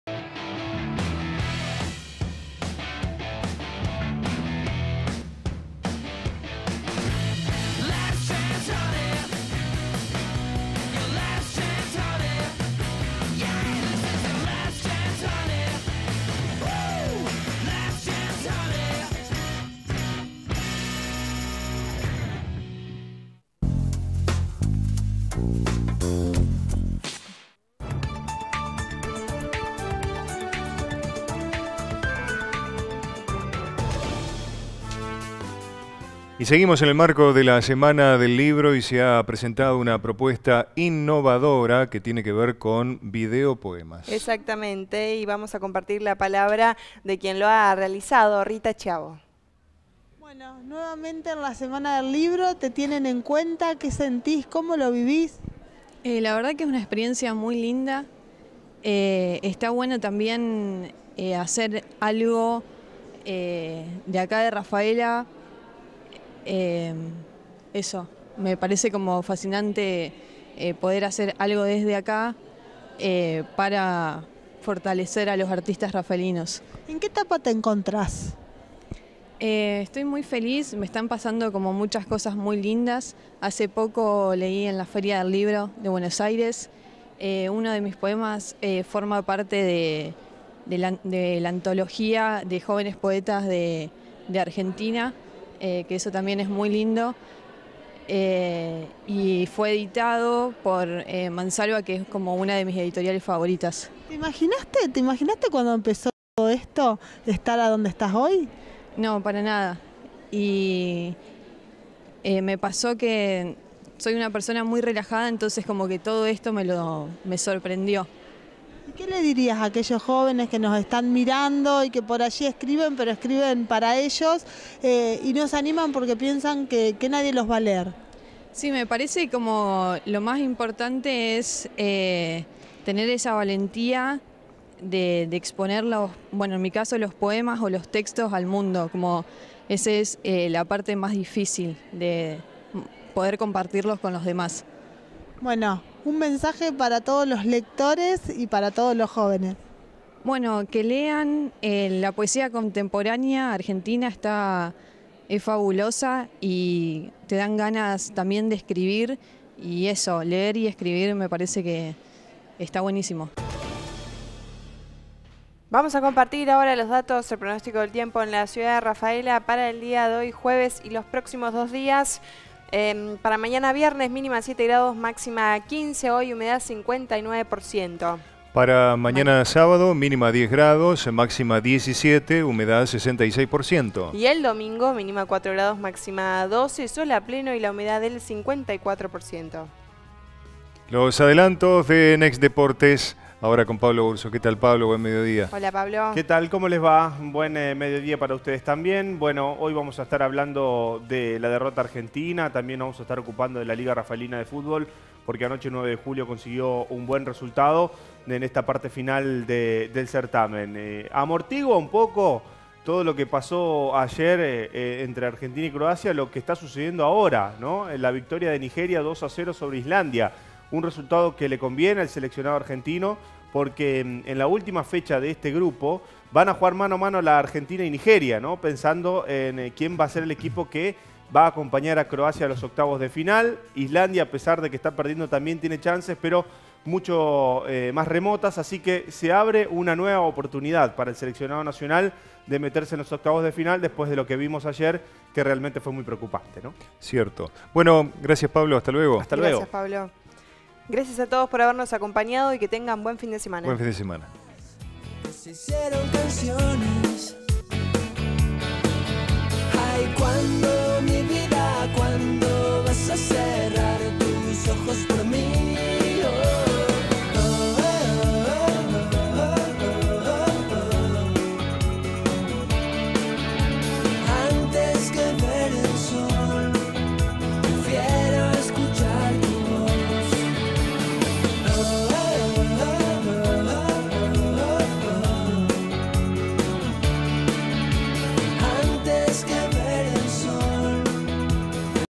Nota Semana del Libro - Next Noticias
Con mucha presencia de público presentó video poemas en una propuesta innovadora